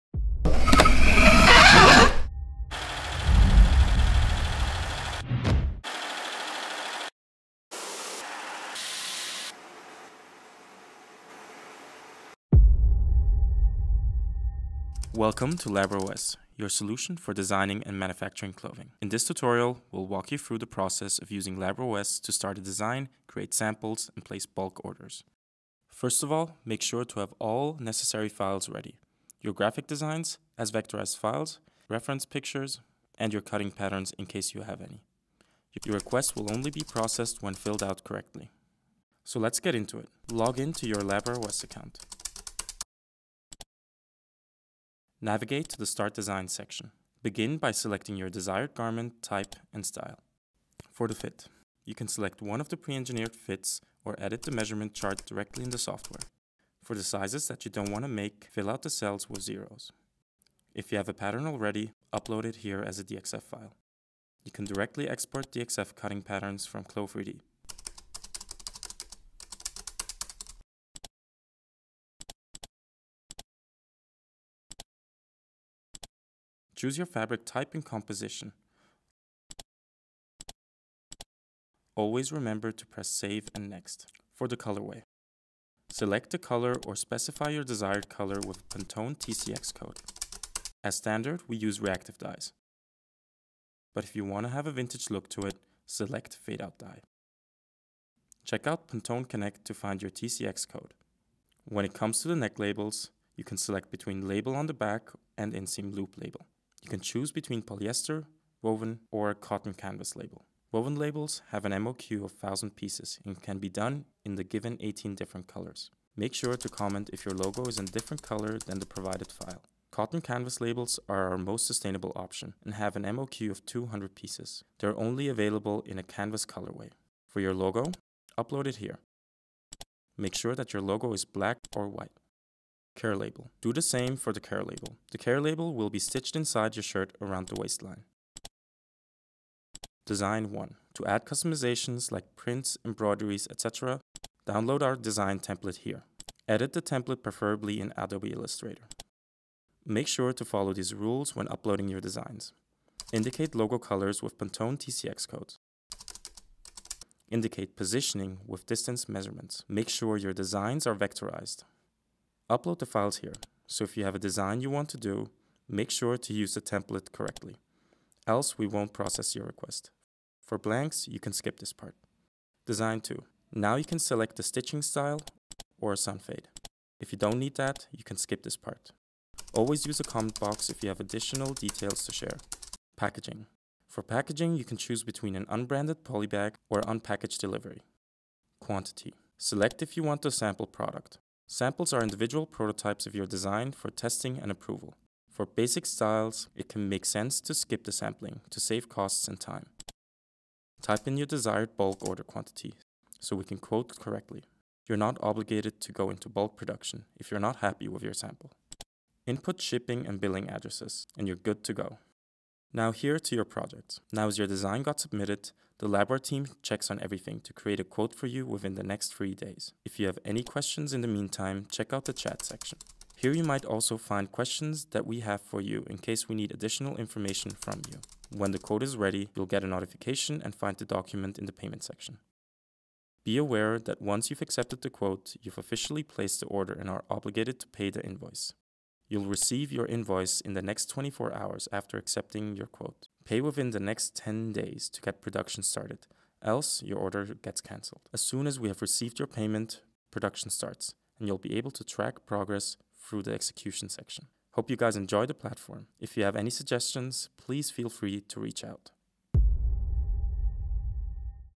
HOW TO MANUFACTURE W/LABWEAR (STEP BY STEP WALKTHROUGH)
Labwear OS is an all-in-one manufacturing platform with an intuitive design flow that makes everything seamless, from creating, to sampling, and tracking production.

In this video, we'll show you how to navigate our software and explain how to:

- Import CLO3D files
- Get your customization files
- Order a sample
- Track your production

With MOQs as low as 50, your pieces will be crafted in top-tier factories in Portugal. Check out our software at os.labwearstudios.com and start transforming your designs into reality!

This is the way to find the right clothing manufacturer.

@labwearstudios on IG and TikTok